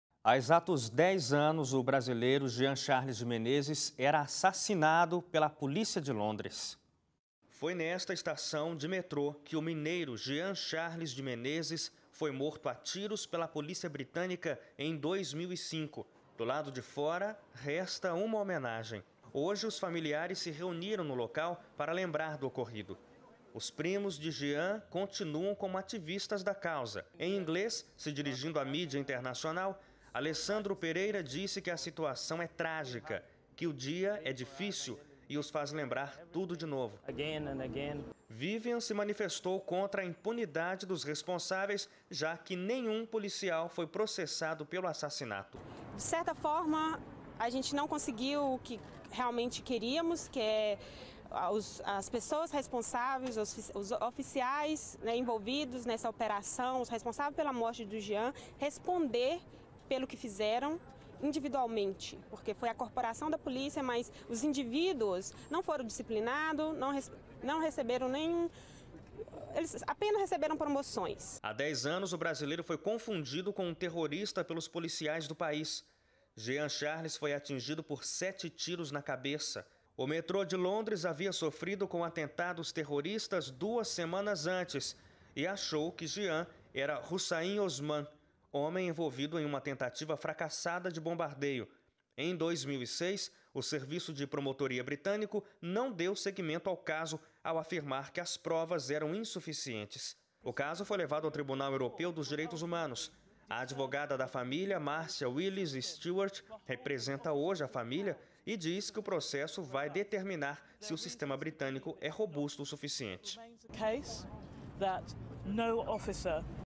Há 10 anos Jean Charles foi morto pela polícia de Londres I Jornal Novo Tempo
Há exatos 10 anos, o brasileiro Jean Charles de Menezes era assassinado pela polícia de Londres.